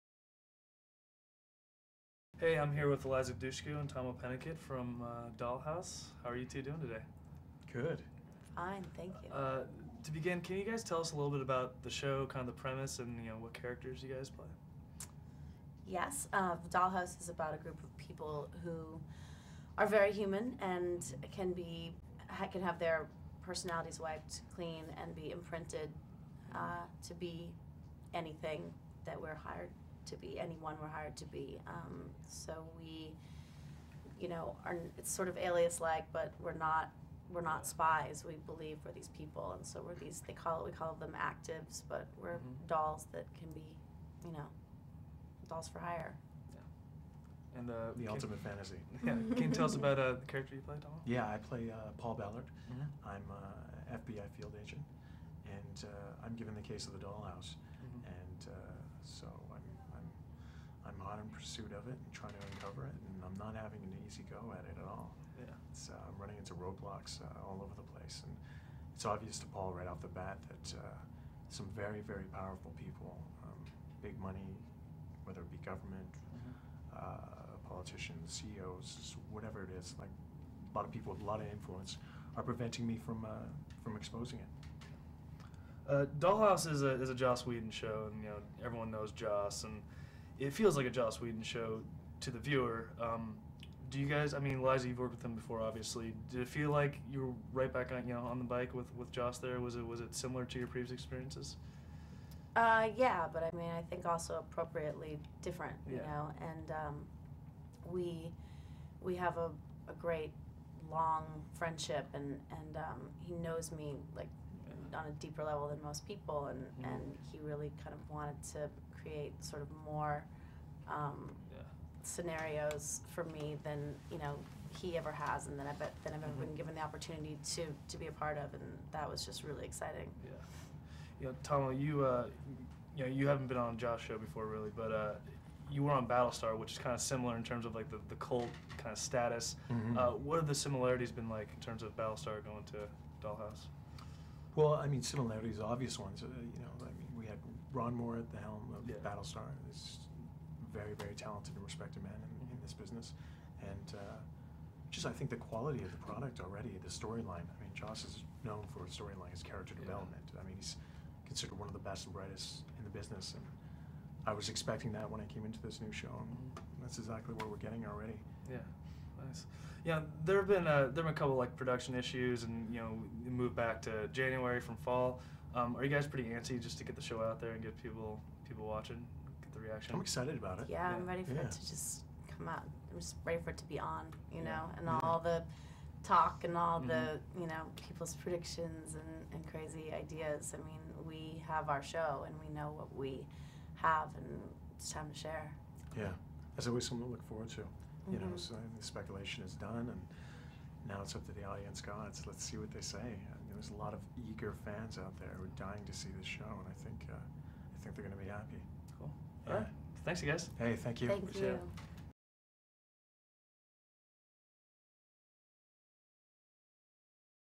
BuddyTV Interview with Eliza Dushku and Tahmoh Penikett
BuddyTV's exclusive interview with Eliza Dushku and Tahmoh Penikett of FOX's new series Dollhouse.